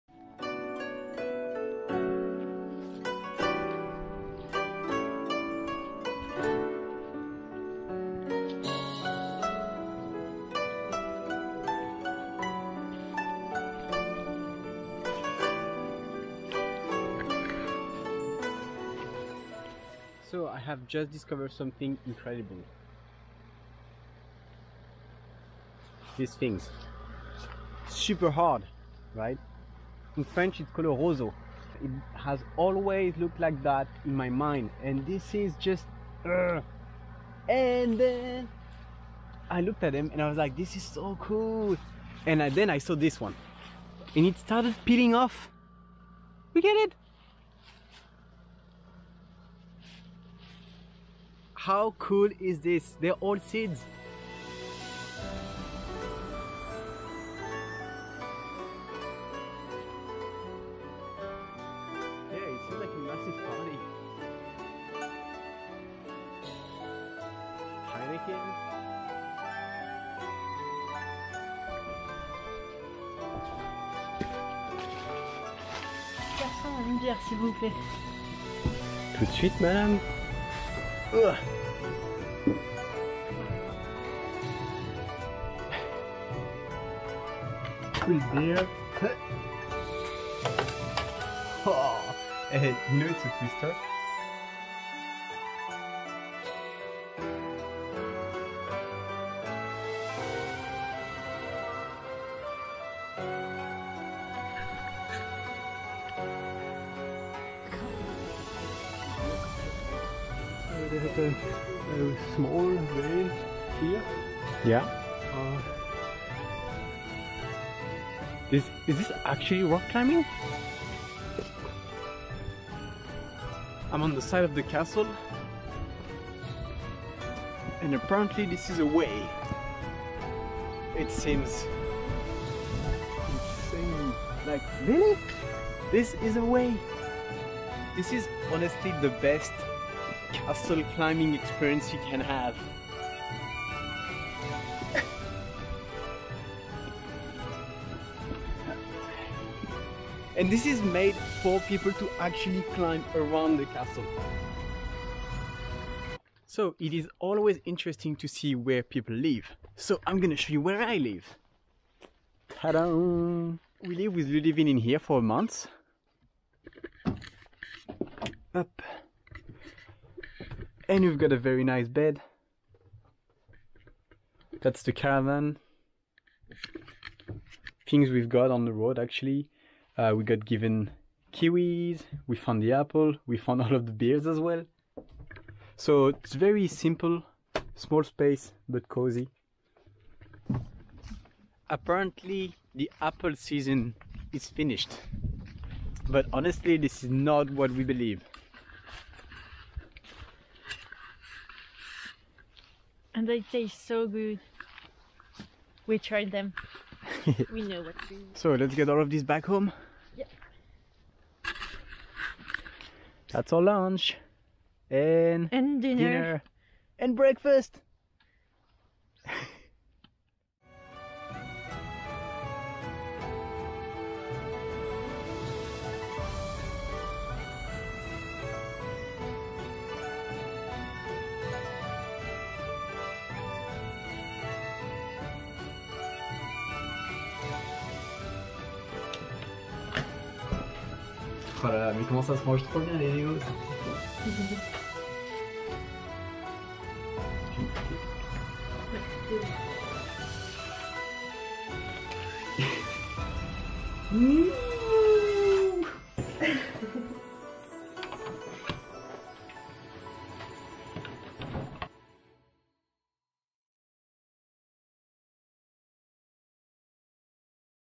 Workaway in Austria at the bogengau
Novembre 2019: Workaway at the archery place, the Bogenbau. We spent a bit less than a months and made so many things
- Bow
- Rabbit fence
- Leather arm guard
- Walk up forest, castles and mountains
- Painted a kitchen
- Saw Graz

Comments & share are always welcome 💝